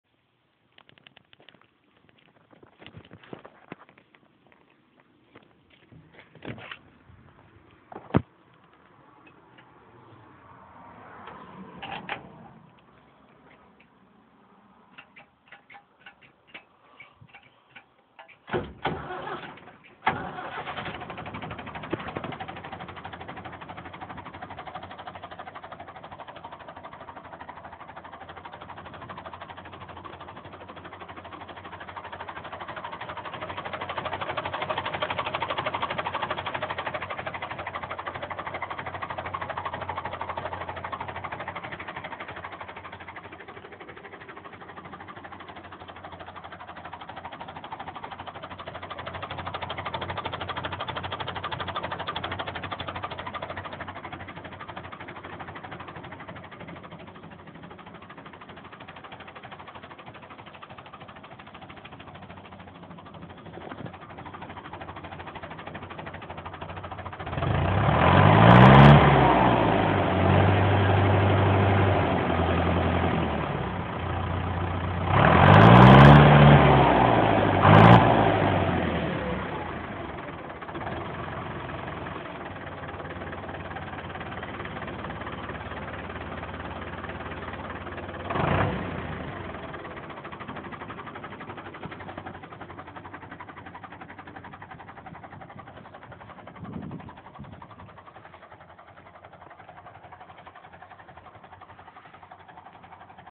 The 990 lives on!
well after 2 weeks of tatting, alot of thought and alot of advice from people, she started, and drove just under 7 miles from my dads to my mums!